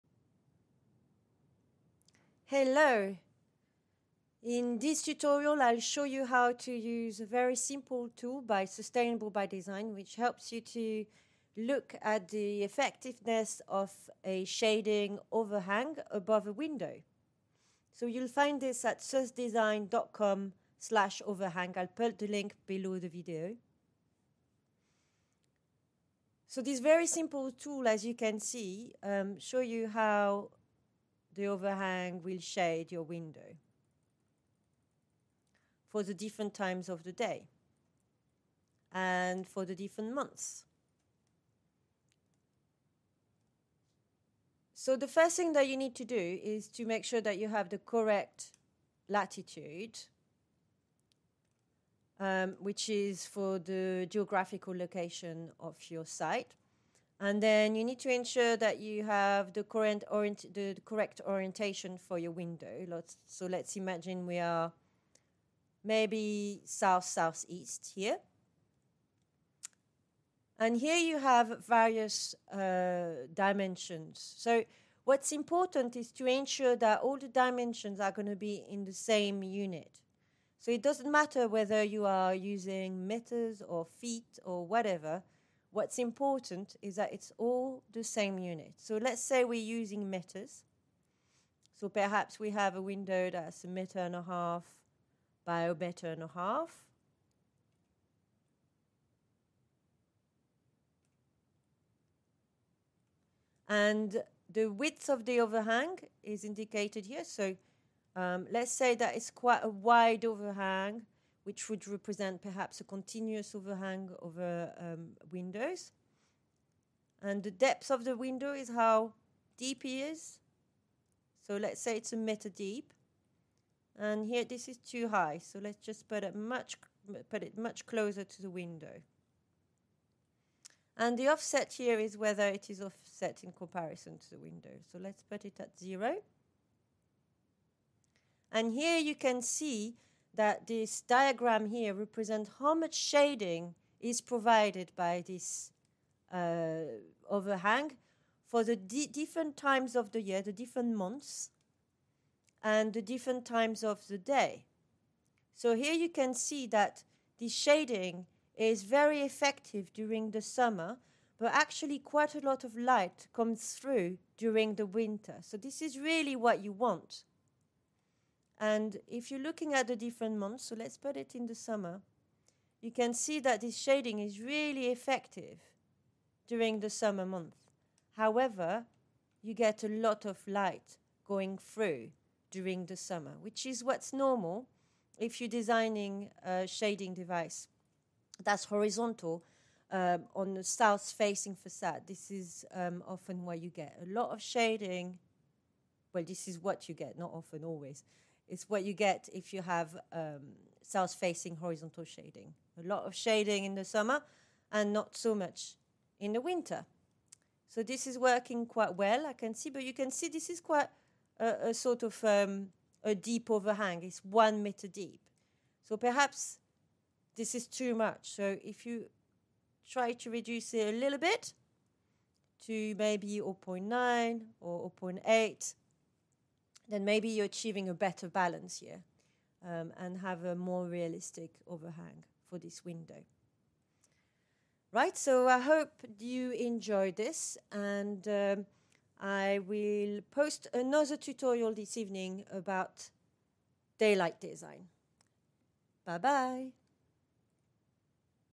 How to design a solar shading overhang for a window.
Hello!
This is a tutorial on how to use a simple online tool to check the shading effectiveness of an overhang using